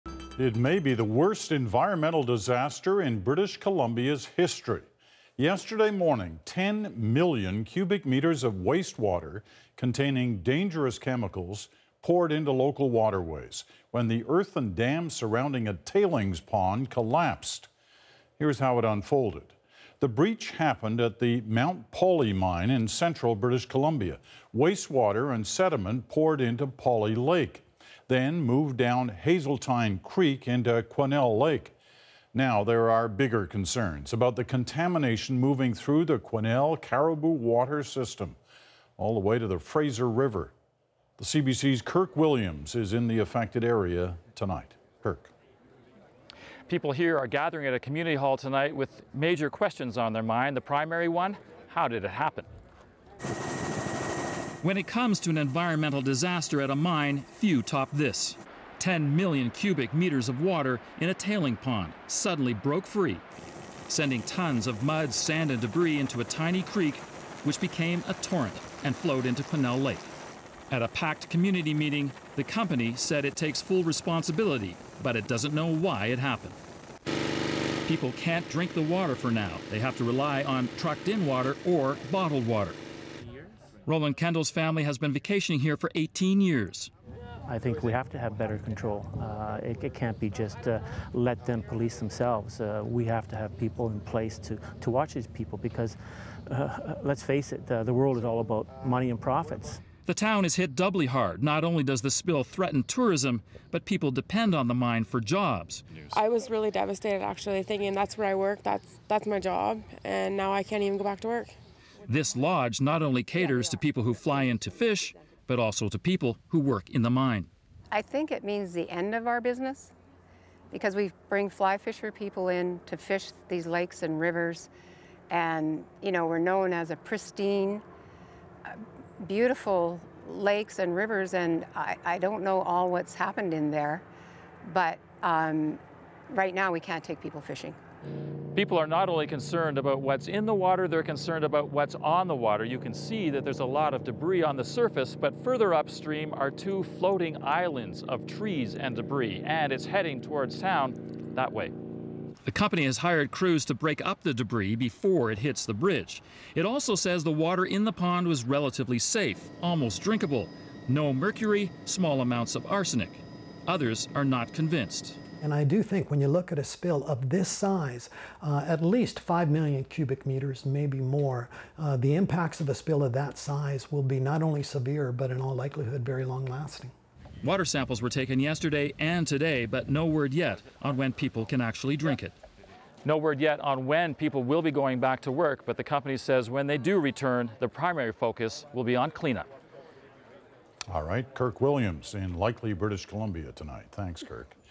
Mount Polley Mine tailings breach
Ten million cubic metres of wastewater containing dangerous chemicals poured into B.C. waterways when a dam surrounding a tailings pond collapsed.